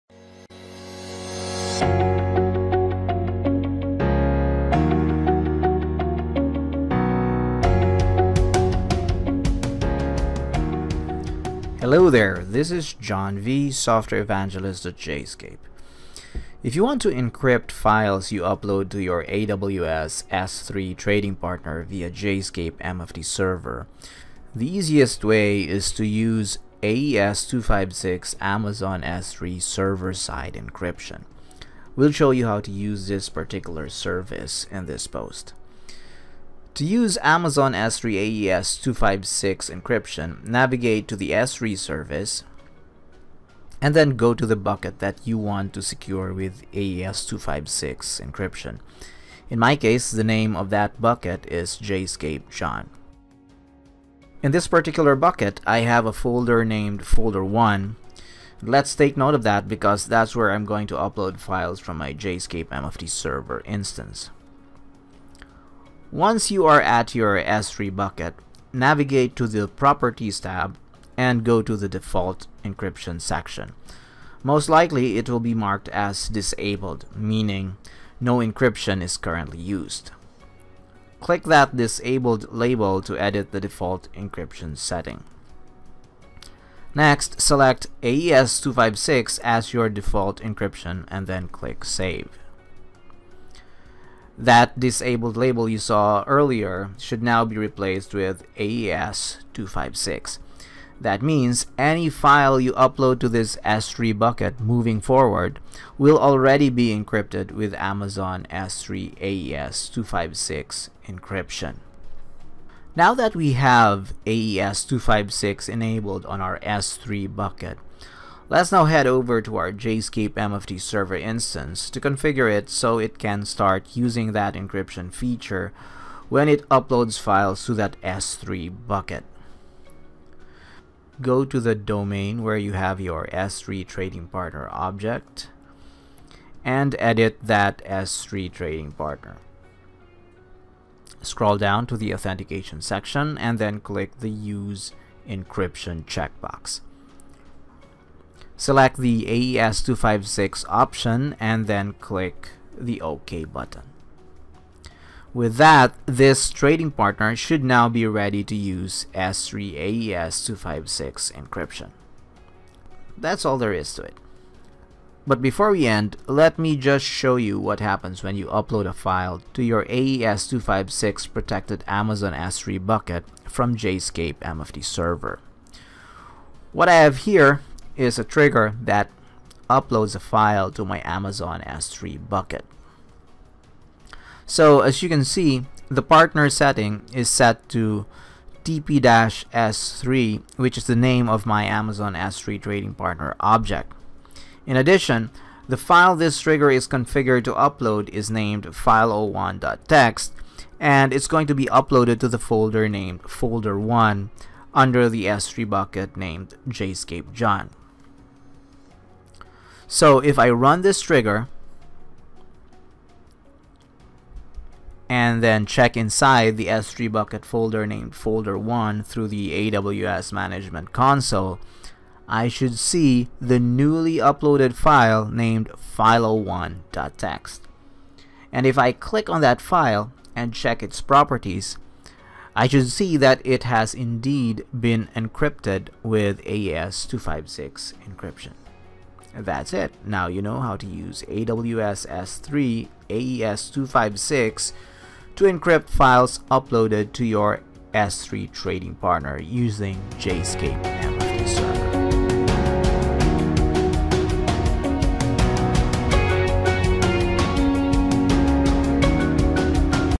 Using AES-256 To Encrypt Files You Upload To Your AWS S3 Folder
Files you upload to an Amazon S3 trading partner through JSCAPE MFT Server are normally stored in plaintext. If you want to encrypt those files to minimize the risk of a data breach, one way to do that is by using AES-256 Amazon S3 server-side encryption. We'll show you how to use this particular service in this post.

Related content:

[article] Using AES-256 To Encrypt Files You Upload To Your S3 Trading Partner

Using AWS KMS To Encrypt Files Uploaded To Your Amazon S3 Trading Partner Through JSCAPE MFT Server